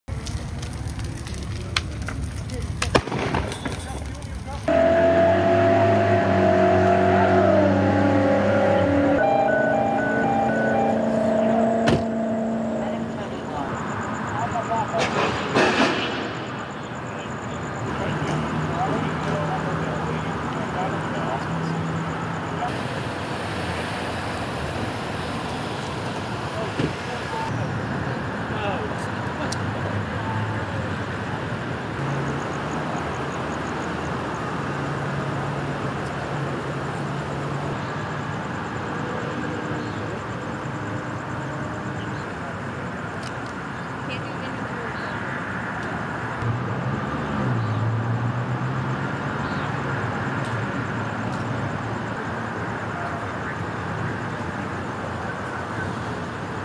Woman dies after car crashes into tree and catches on fire
9-29-24 | 3:49 a.m. | 4900 Fondren